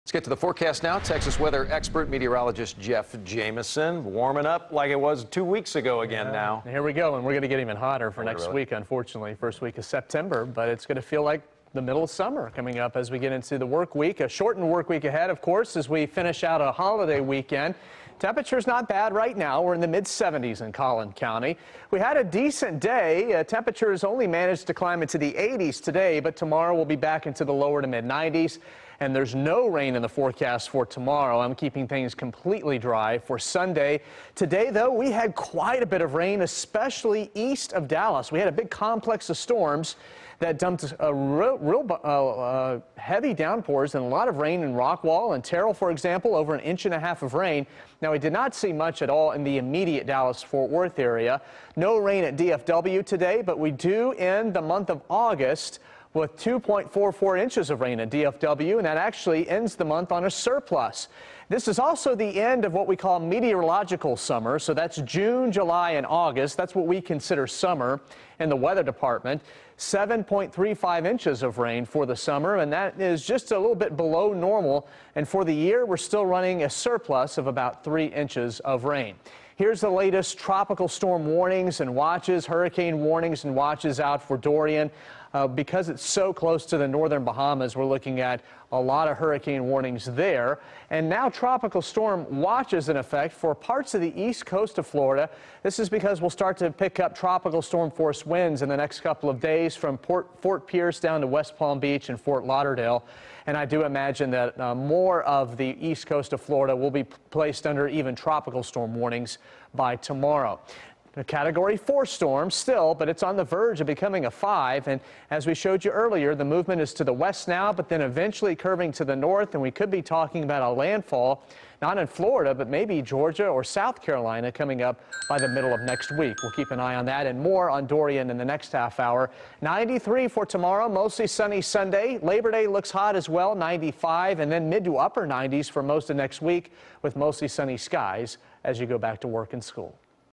Jeff Jamison's Weather Forecast
North Texas has a warmer week ahead with temperatures expected to remain in the mid-to-high 90s.